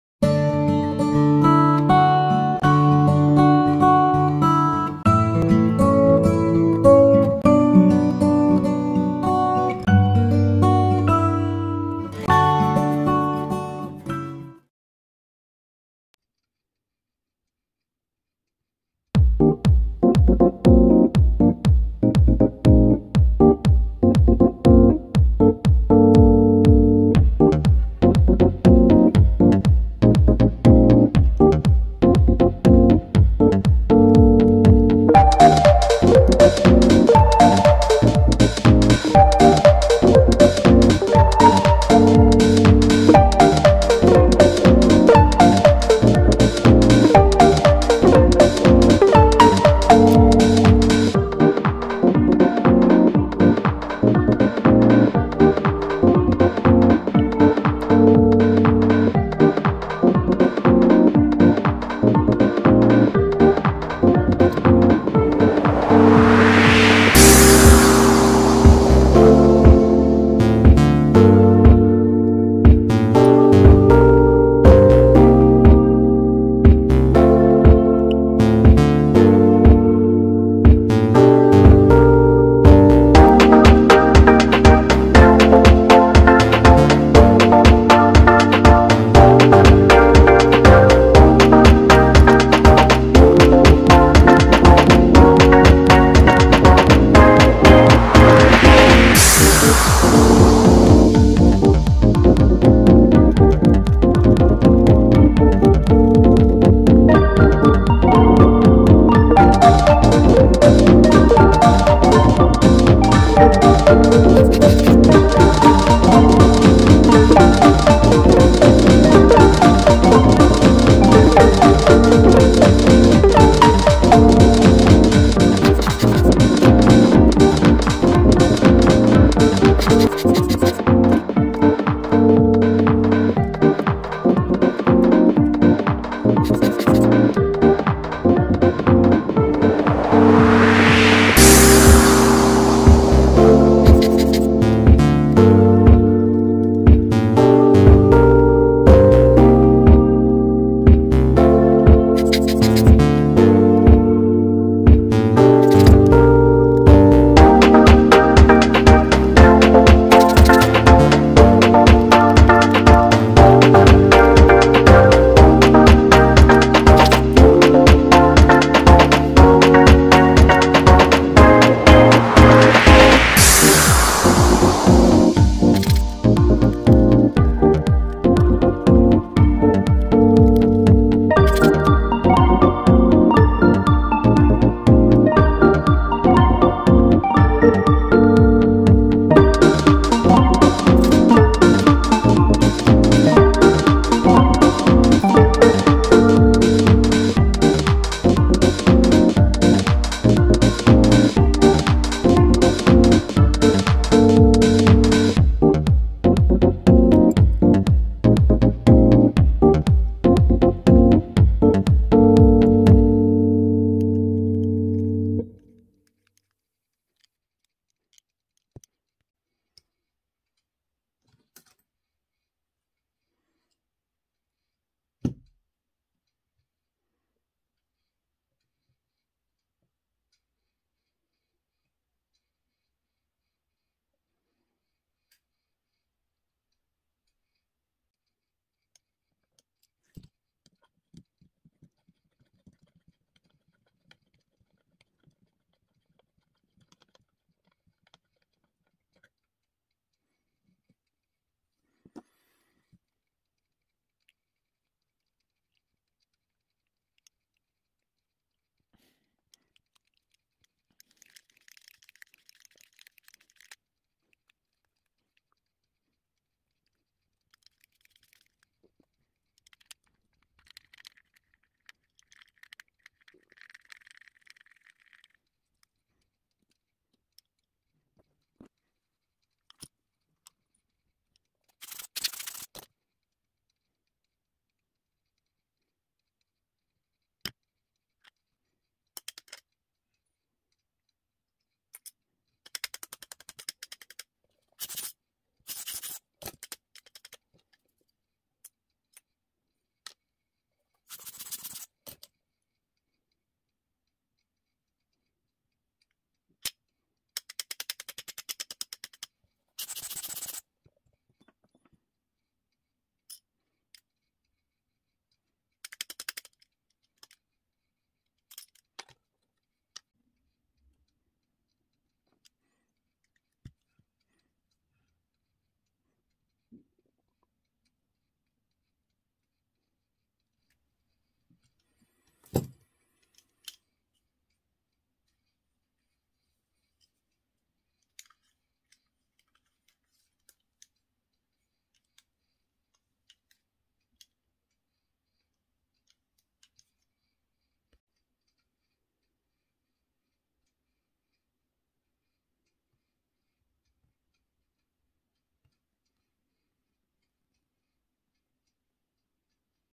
헬리오스 44-2 조리개날 기름제거, 어렵지 않아요~
헬리오스 44-2는 대부분 조리개날에 기름이 있습니다.
완전히 분해하지 않고 기름을 제거하는 법을 소개합니다.
주의할 점은 힘주지 말고 살살해야 합니다.